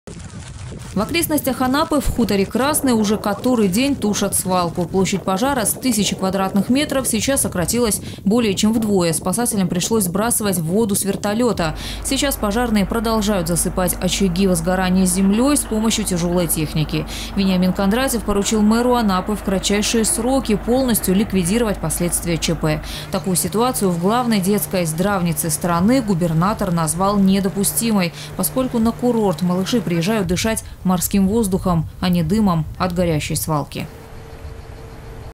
В Анапе продолжается ликвидация тления на полигоне ТБО. Новости Сочи Эфкате
Губернатор поручил разобраться с последствиями в кратчайшие сроки.
Сочи. Новости.
Телеканал Эфкате.